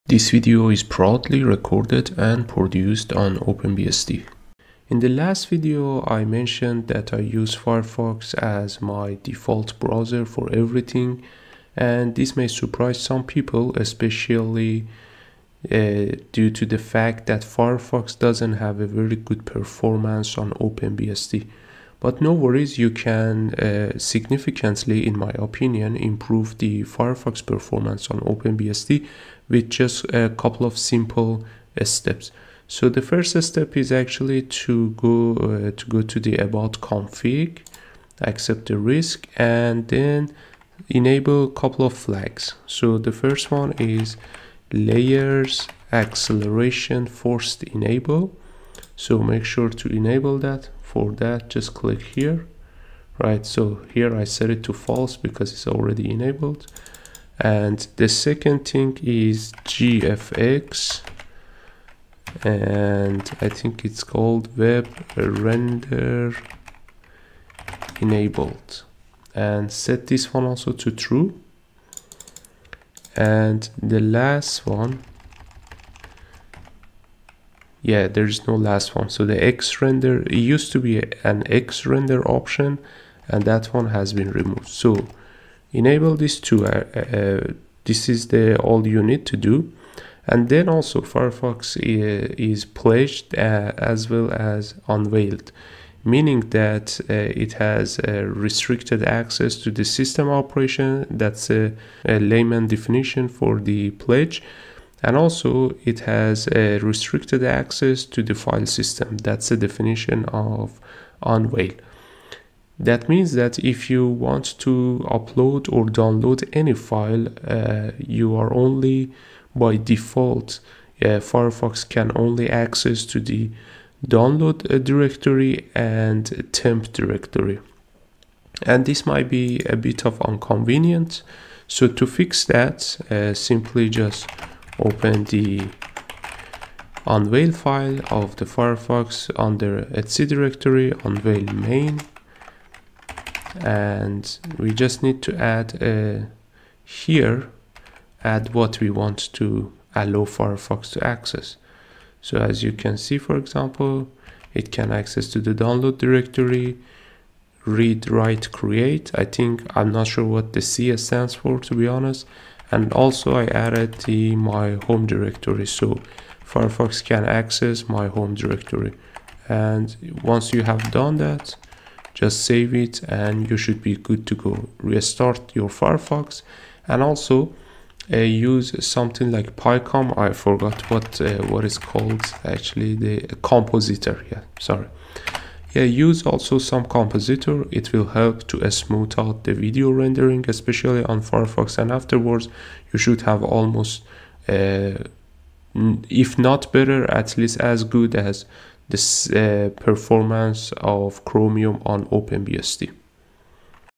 Simple Firefox performance optimizations on OpenBSD
Get much better Firefox performance on OpenBSD by enabling only two flags.
Source: cat /usr/local/share/doc/pkg-readmes/firefox